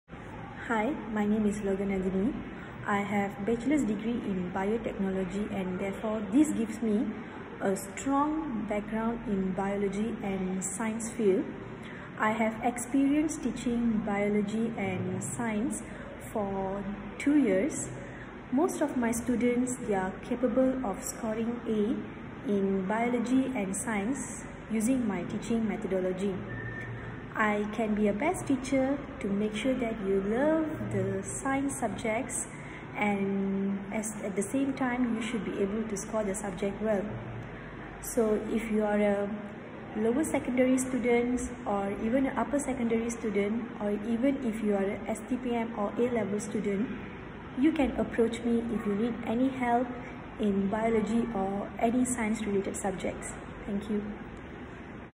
Loganantini Paramasivam teaches Science, Biology, Chemistry near Ulu Tiram, Johor Bahru.
Loganantini Paramasivam teaches Science, Biology, Chemistry near Ulu Tiram, Johor Bahru. She has more than 2 years of teaching experience.